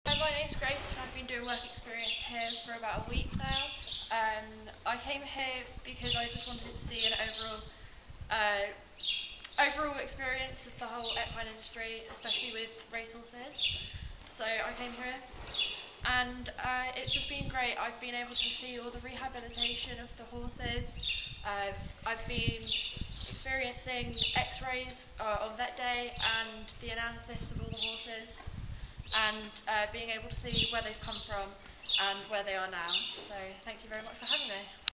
Grace - Level 3 Equine student at Sparsholt College.
Grace is a Level 3 Equine student at Sparsholt College. She attended a one week live in placement at BTRC.
If you would like to find out more about Work Placements at BTRC, please